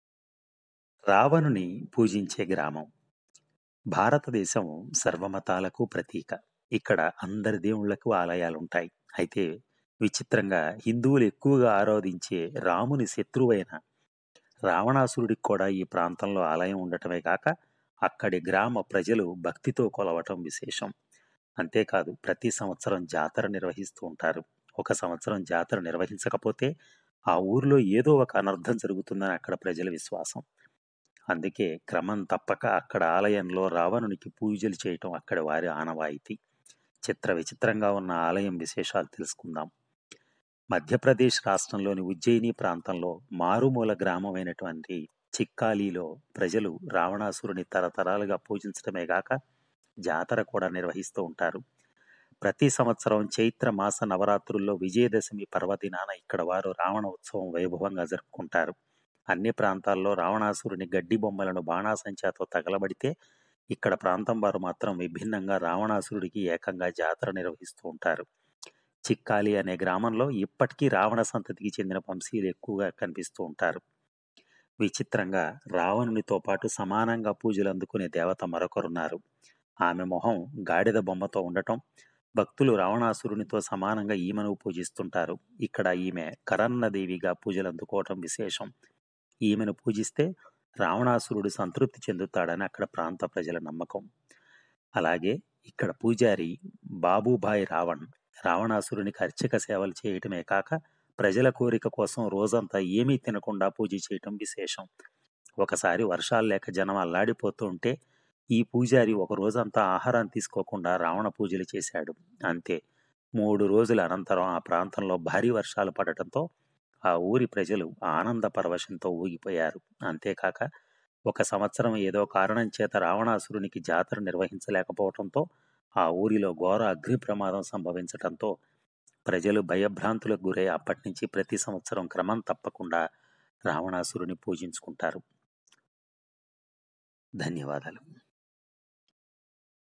రావణాసురుడిని పూజించే గ్రామం. ఒక్క సంవత్సరం రావణ జాతర చేయకపోతే ఆ ఊరికి అరిష్టం.వినండి ఆ ఊరు గురించి
రావణ ఆలయం